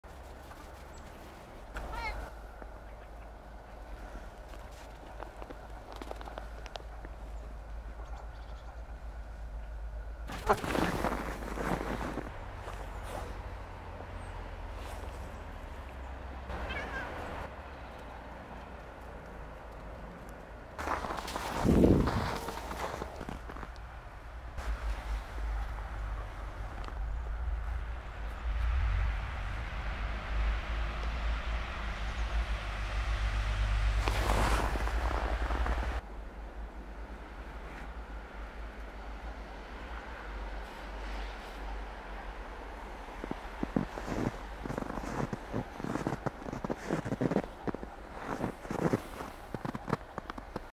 Rei's Snowy Sprint & Hilarious Serenade! 🏃‍♀️❄️
Watch in awe as Rei The Cat dashes through the snowy landscape in a fur-tastic sprint, leaving a trail of laughter and joy! 🌨️🐾 Brace yourselves for a symphony of adorable and funny sounds that will warm your heart in the winter chill. 😹🎶 From comical meows to playful purrs, Rei turns the snowy scenery into her personal playground of hilarity. 🌟💕

Follow Rei on Instagram: reithekittycat